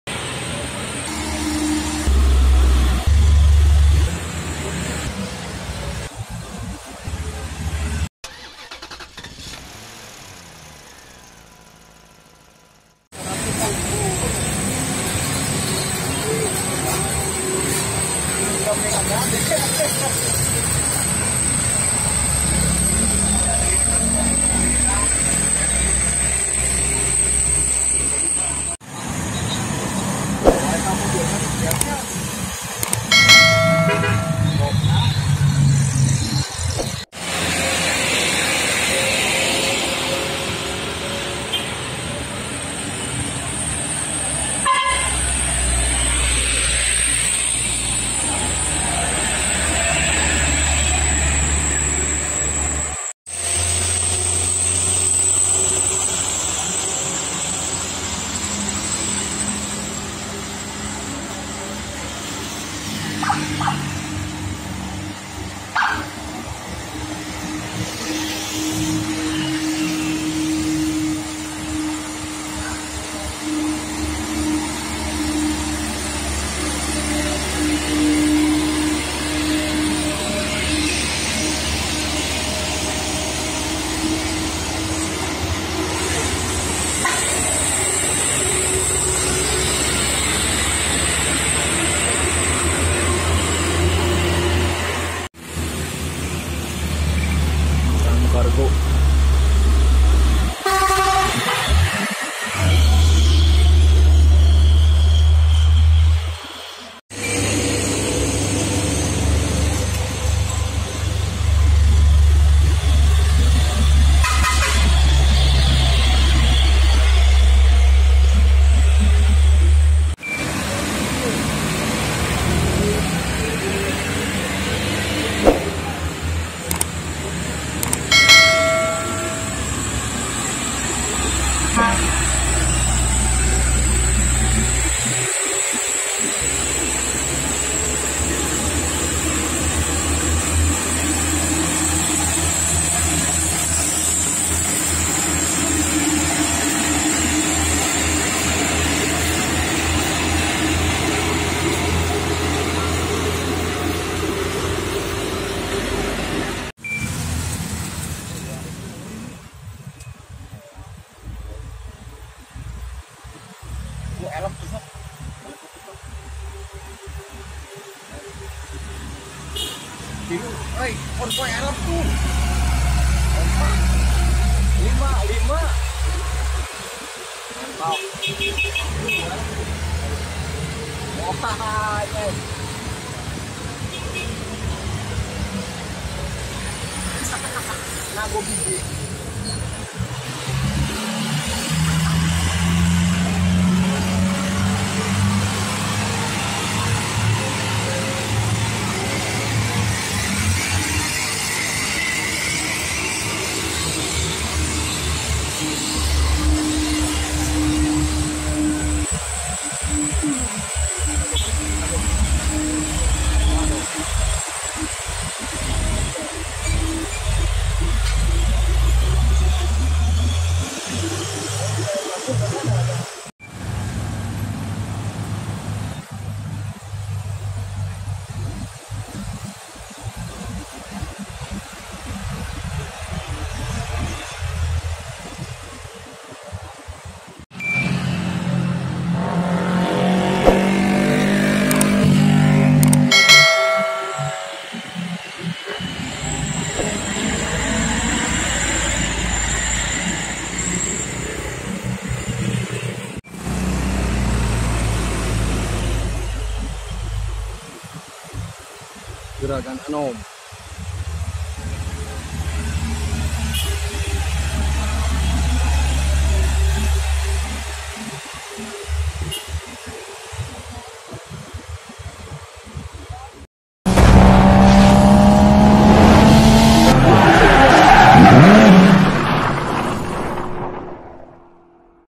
Konvoi 5 Mobil Elf Pariwisata, Saling Kejar-Kejaran Ditanjakan Batununggul, Mbois Oleng Muatan Berat
LOKASI: TANJAKAN BATUNUNGGUL, KABUPATEN CIAMIS, JAWA BARAT